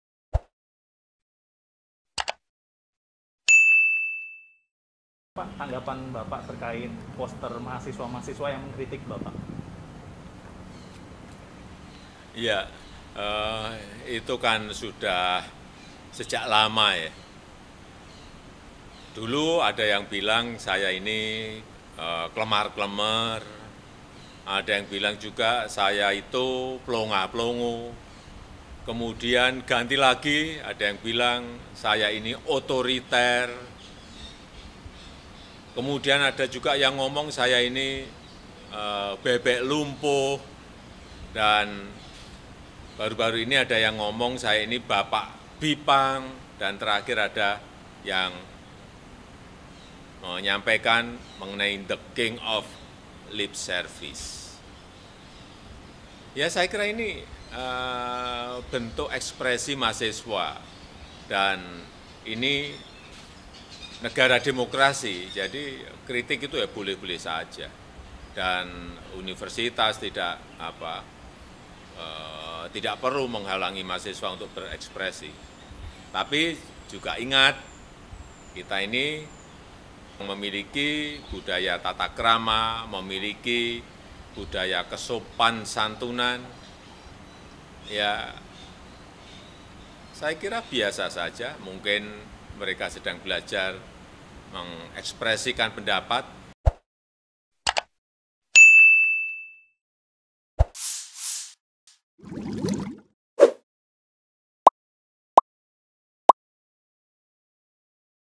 Jokowi Tanggapi Kritikan BEM UI, Juluki 'The King of Lip Service'
Presiden Joko Widodo menanggapi kritikan Badan Eksekutif Mahasiswa (BEM) Universitas Indonesia (UI) yang juluki Jokowi The King of Lip Service.

Tanggapan tersebut disampaikan Jokowi melalui video yang diunggah di akun Resmi Sekretariat Presiden, Selasa (29/6/2021).

Video terkini dan terbaru..